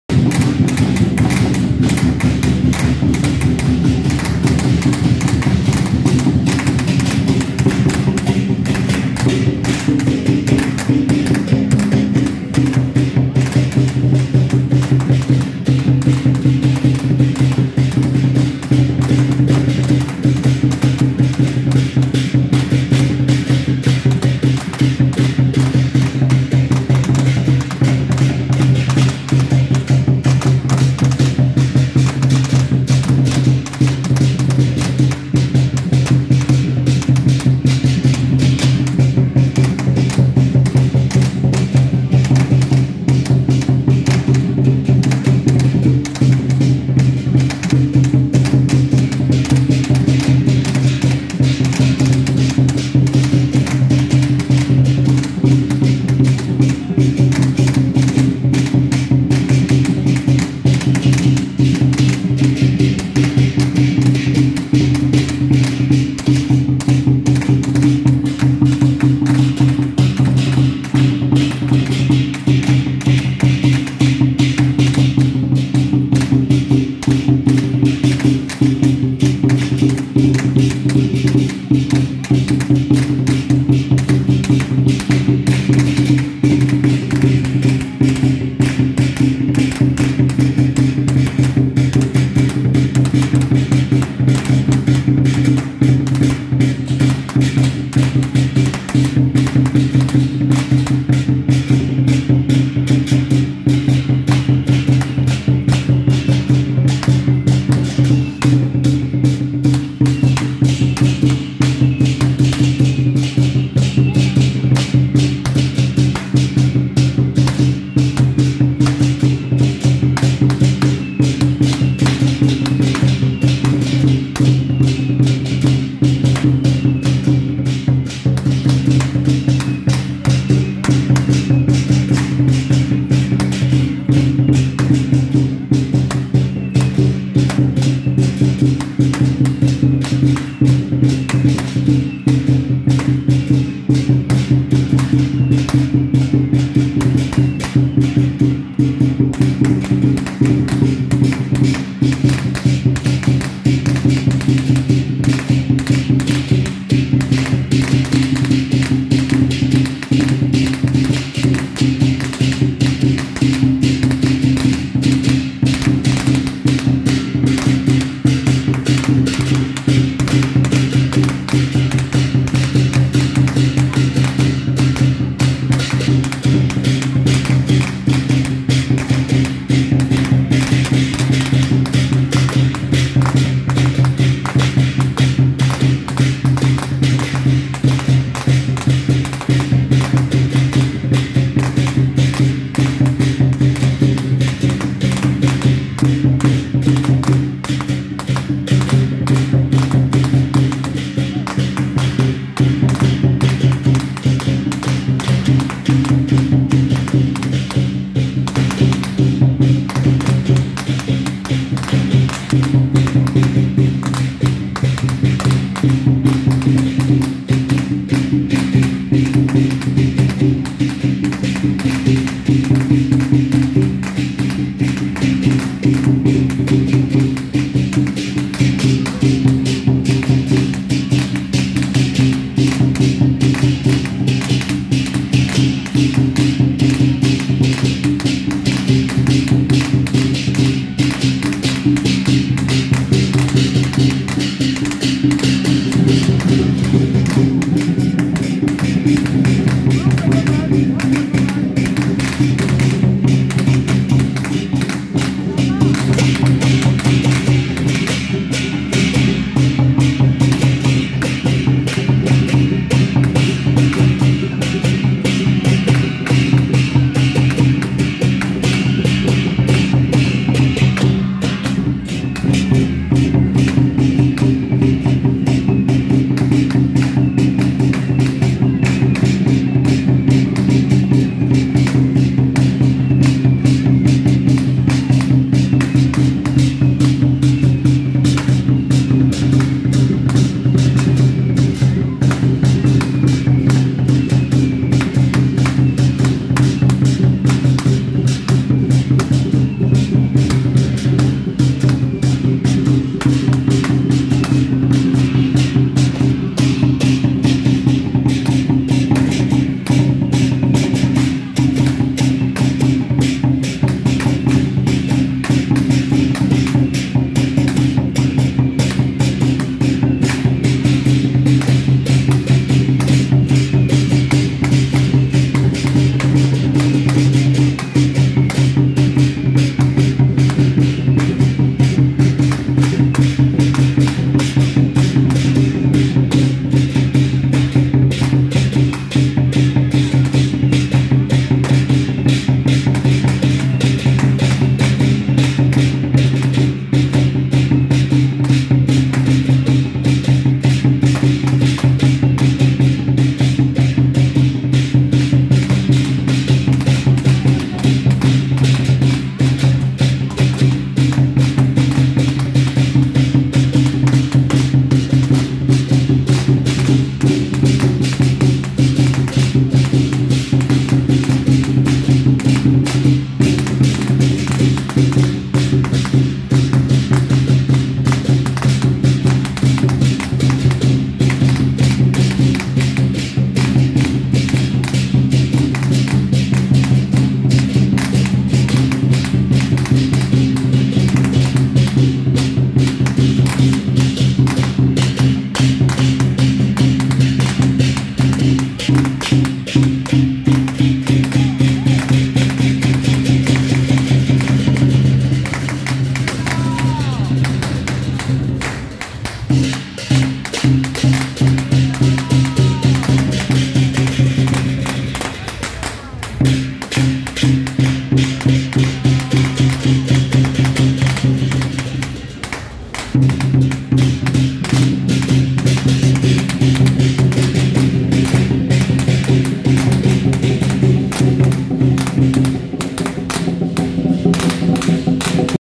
【大溪迎富送窮廟-神將團＆大溪九壽山無極圓修宮-鴻鈞聖祖 聖駕】~111大溪三層福安宮恭迎天上聖母遶境
大溪迎富送窮廟-神將團＆大溪九壽山無極圓修宮-鴻鈞聖祖 聖駕